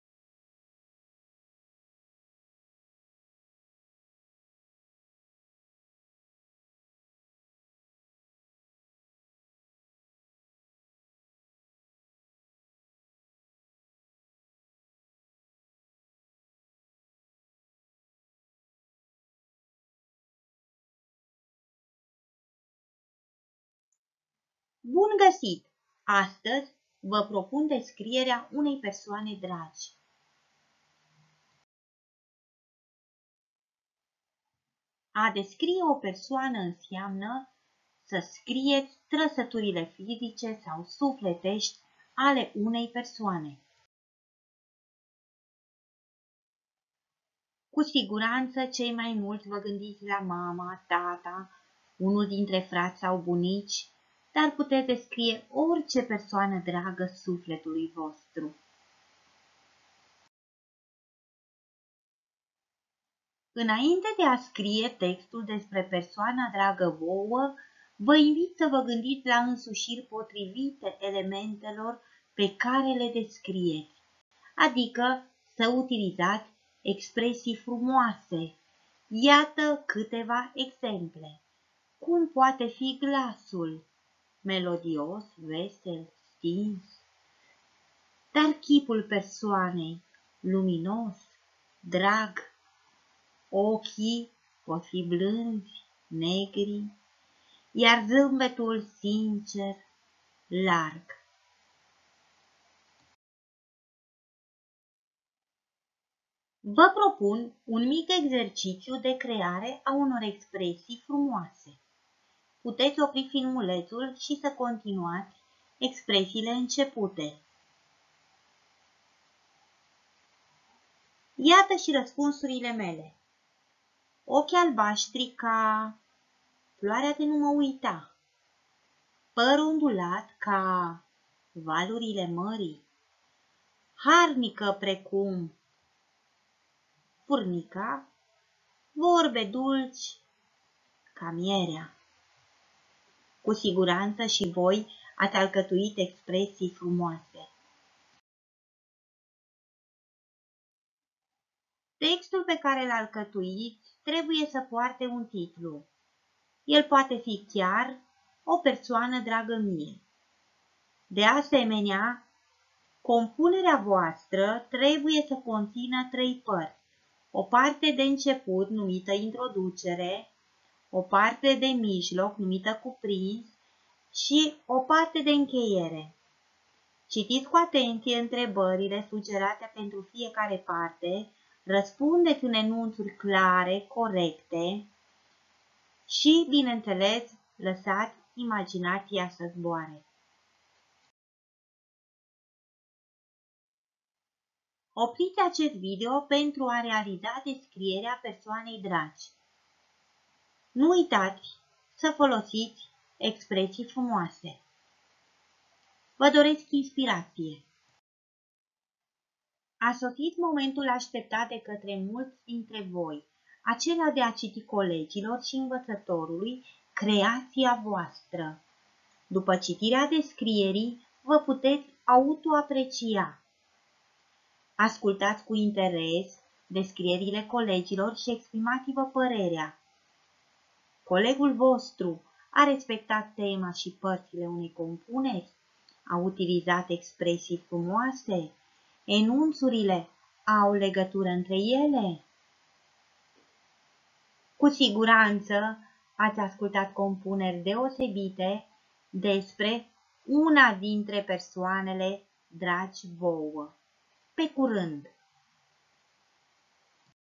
Descrierea unei persoane dragi
Resursa propune elevilor sugestii de alcătuire a unei descrieri.
Elevii au libertatea de a-și alege persoana pe care o vor descrie.
Se oferă sugestii de compunere a unor expresii frumoase și planul compunerii sub formă de întrebări. La finalul resursei elevii sunt îndrumați în autoevaluare și în aprecierea creațiilor colegilor.

Autor: Elisabeta Maria Mînecuță